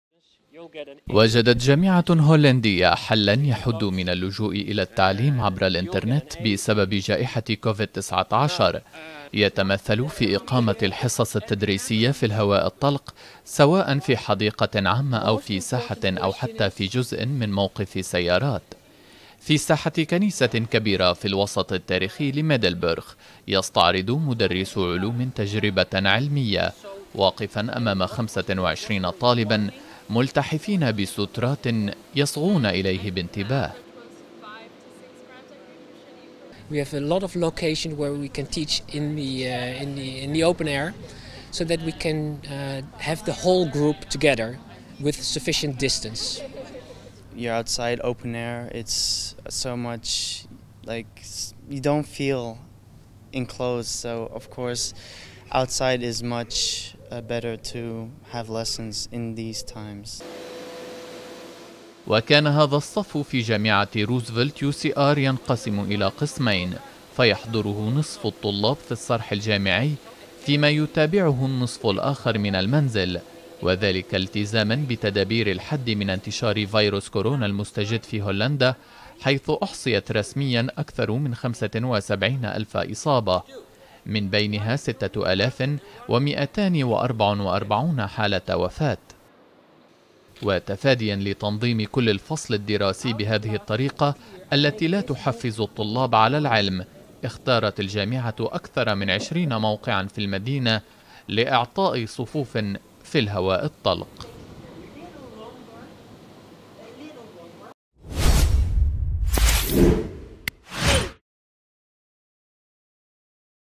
صفوف في الهواء الطلق لكسر رتابة التعليم من بعد في هولندا
وجدت جامعة هولندية حلّاً يحدّ من اللجوء إلى التعليم عبر الإنترنت بسبب جائحة كوفيد-19، يتمثل في إقامة الحصص التدريسية في الهواء الطلق، سواء في حديقة عامة أو في ساحة أو حتى في جزء من موقف سيارات.